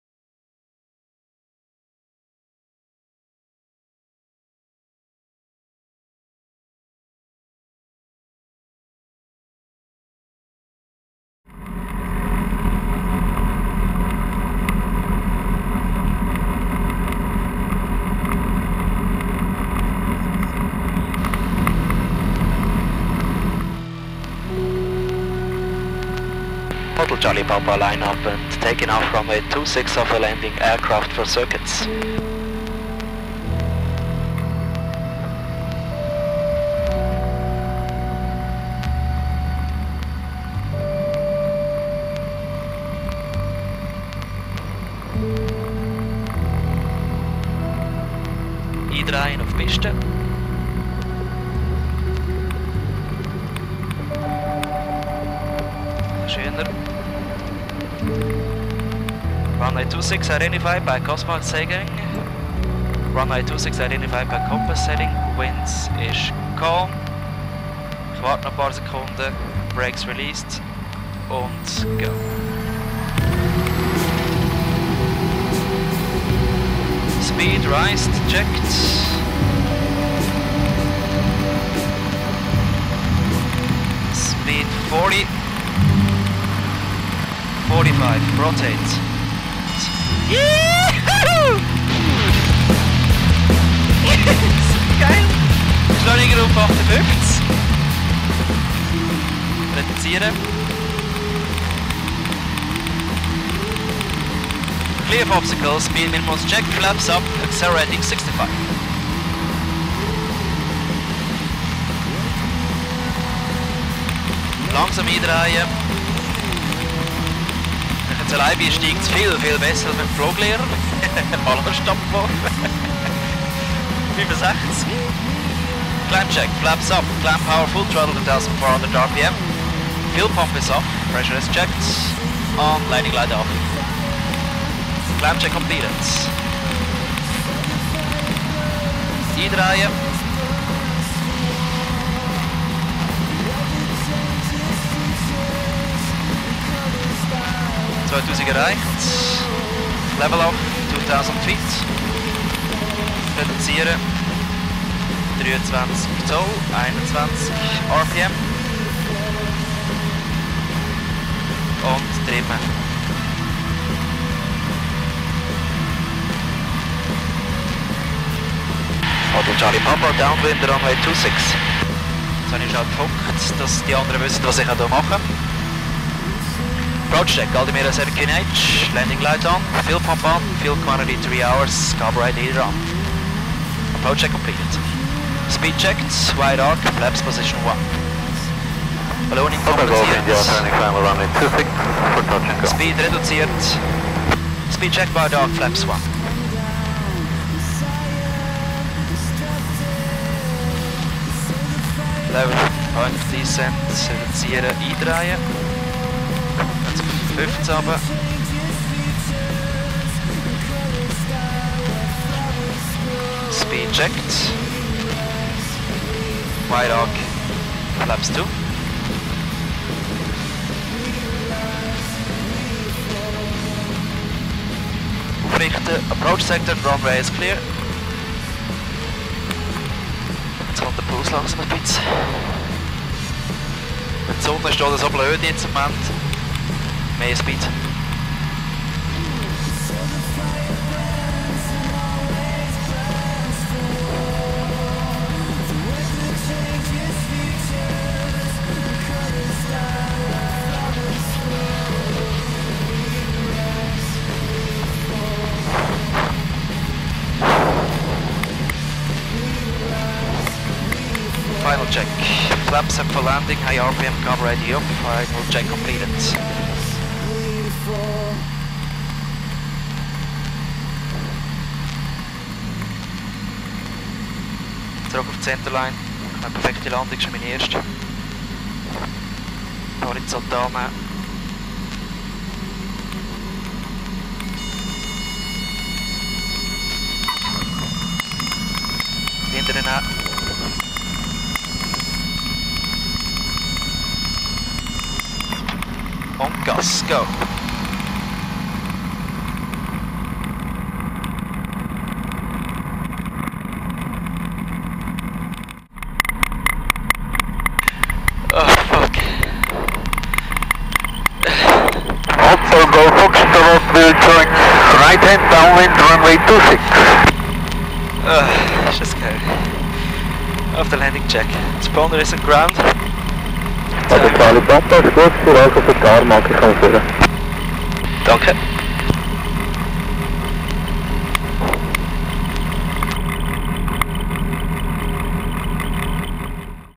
Erster Soloflug mit der DV20 in LSZF
Ein unbeschreibliches Erlebnis für jeden Piloten! Das erste Mal alleine am Steuer. Klar dass da der Puls ansteigt (was man am Anfang auch merkt) Der Flug wurde für eine Radiosendung so weit wie möglich kommentiert.
Der erste Solo bestand komplett aus drei Volten.
Zu sehen ist die erste Volte und am Schluss noch die Emotionen nach der letzten :-)

Flugzeug: Diamond Katana DV20
Platz: Birrfeld (CH) LSZF